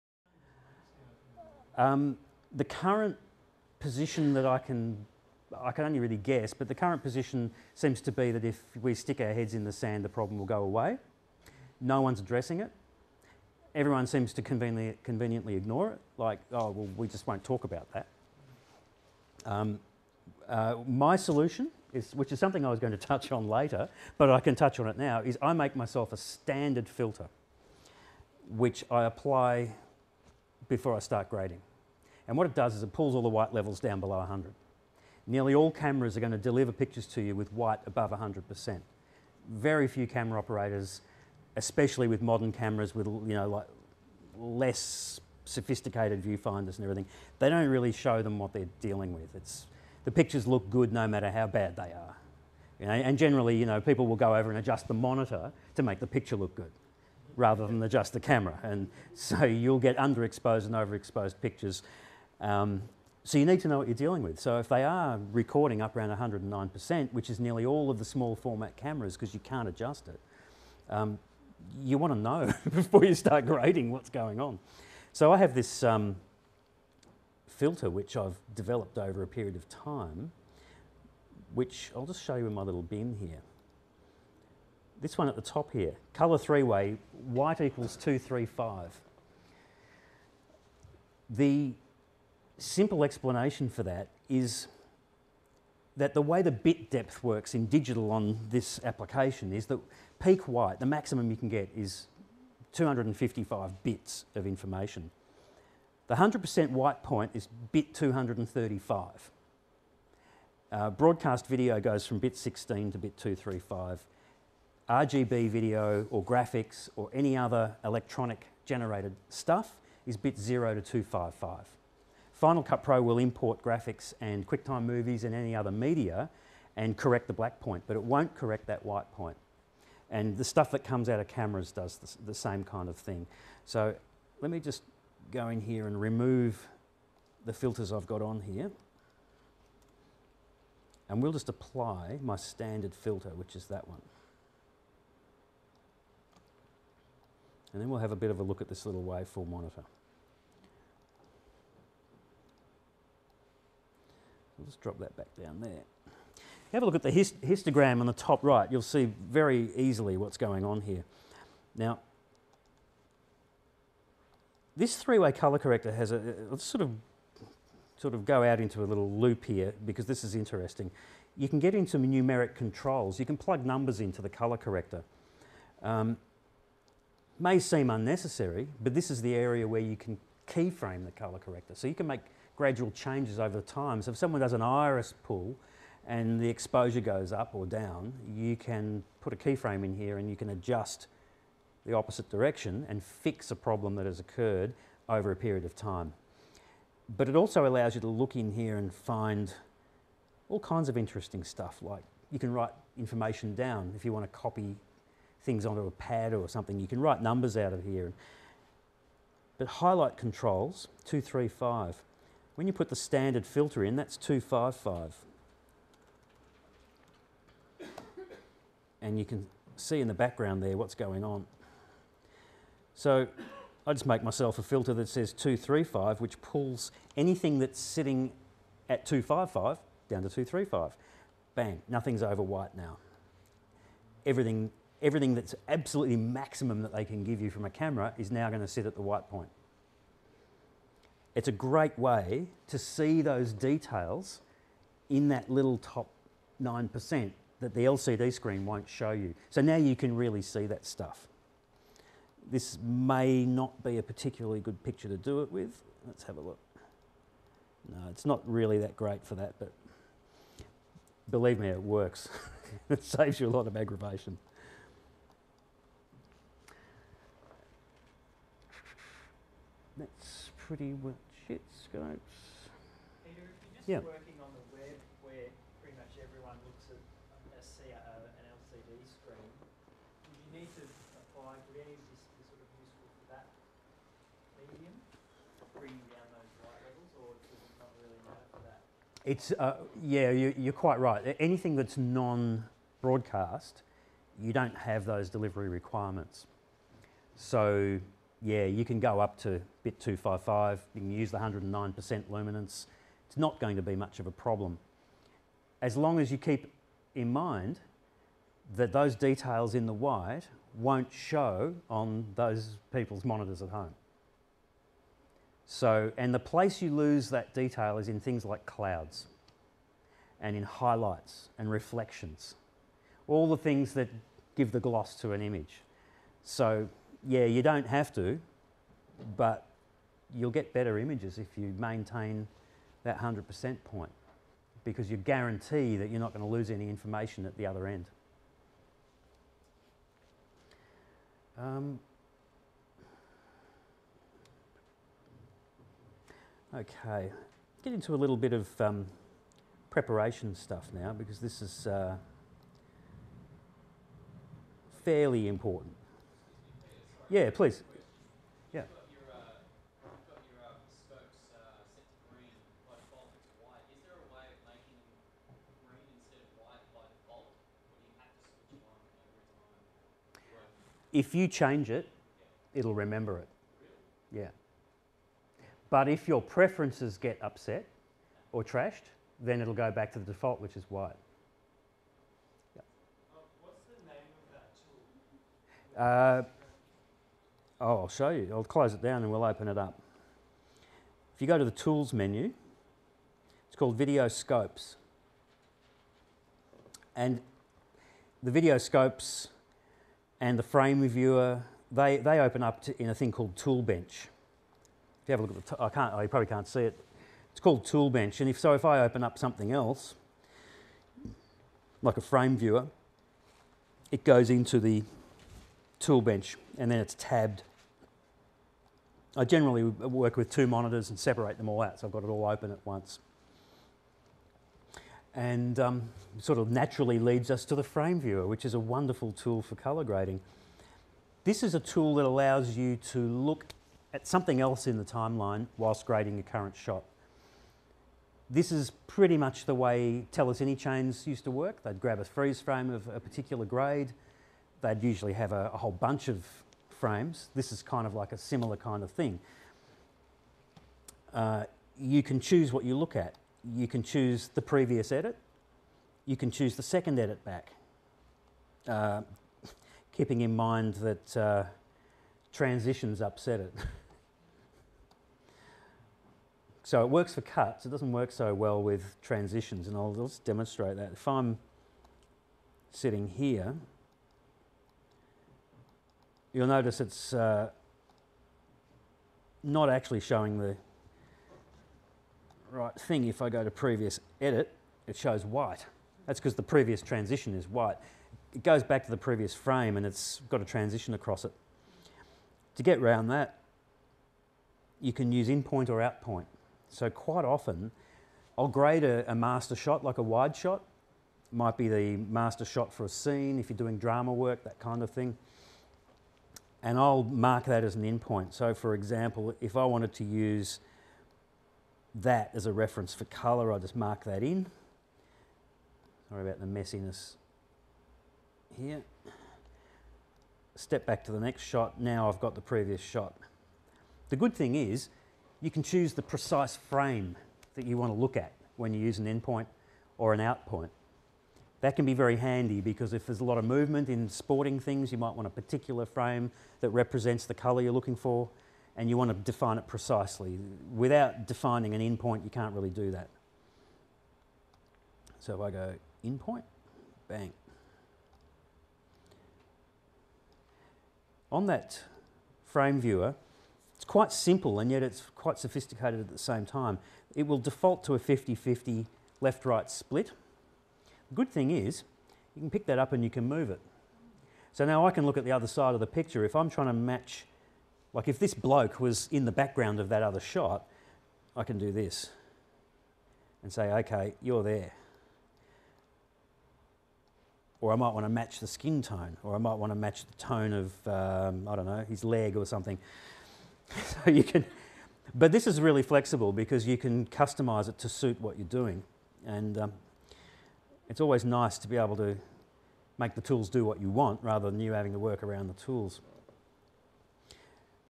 Final Cut Pro Colour Grading Seminar by Peter Cave (Part 5 of 9)
Australian Screen Editors presents "Making The Grade", a seminar on colour grading in FCP by Peter Cave. This is part 5 of 9.

Topics covered in this part include: White Point Preset Filter, Numeric Color Corrector Control, Keyframing, Tool Bench, Frame Viewer, Match Hue.

Peter Cave is a colourist with 20 years freelance experience. Special thanks to Swinburne University School of Film &TV for hosting and filming the event on 13 July 2009.